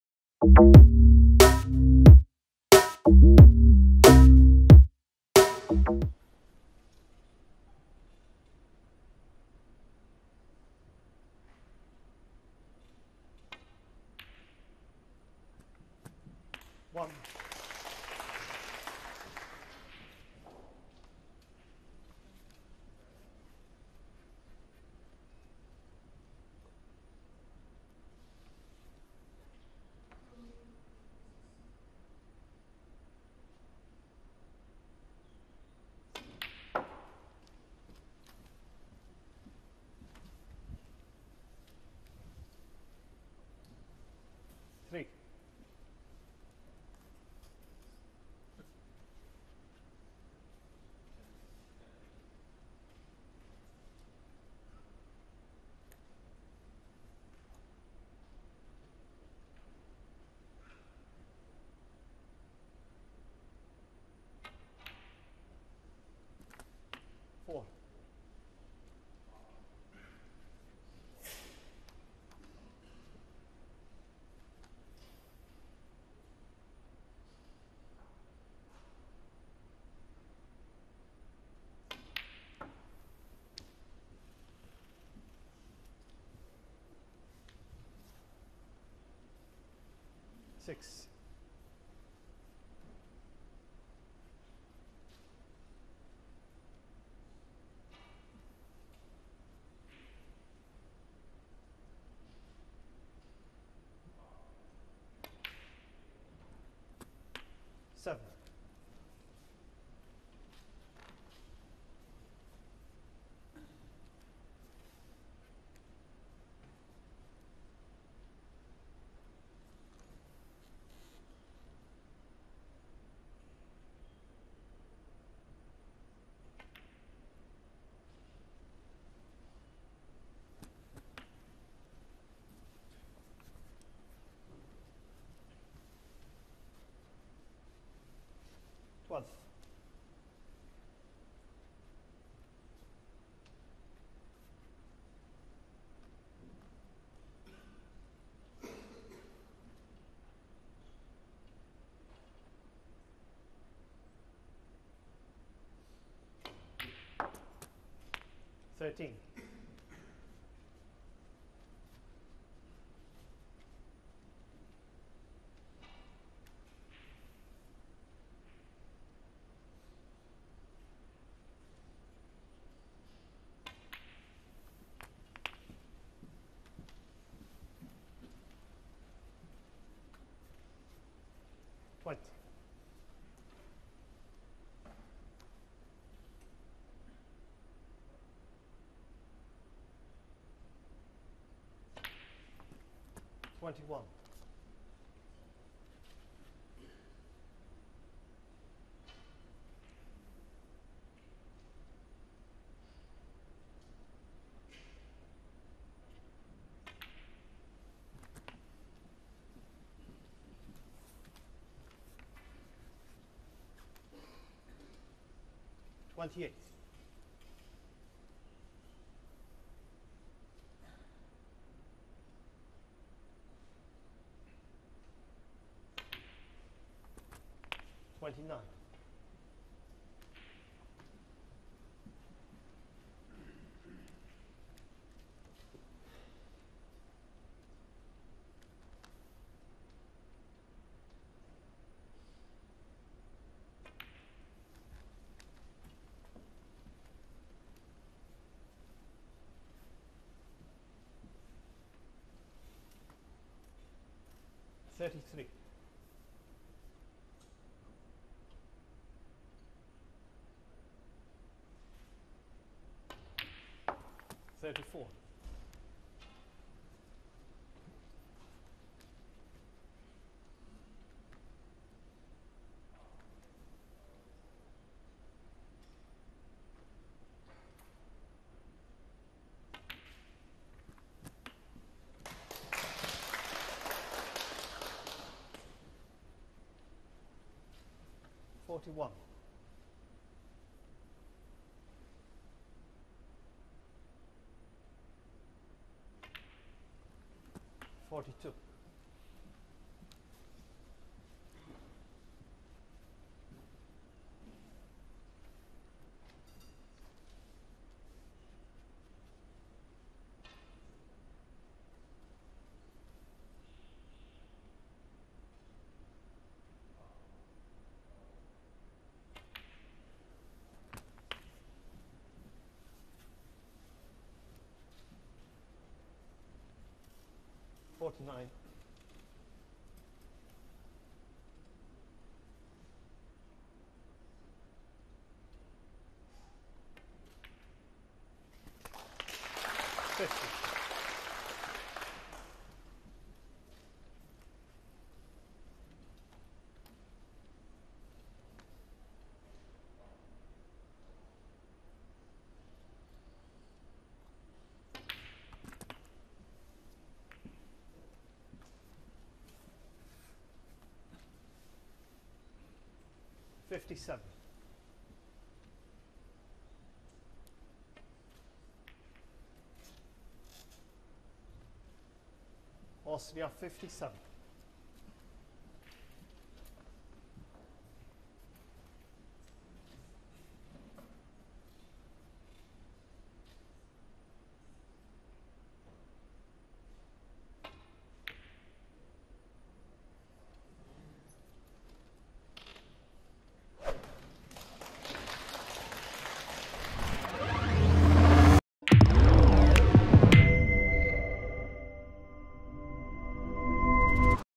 Ones To Watch | Florian Nüßle
You’d be forgiven for any nerves knowing that Ding Junhui is ready to mop up from a single mistake you make…

But Austrian hotshot Florian Nuessle showed nerves of steel with this classy frame-winning contribution at the World Cup 🇦🇹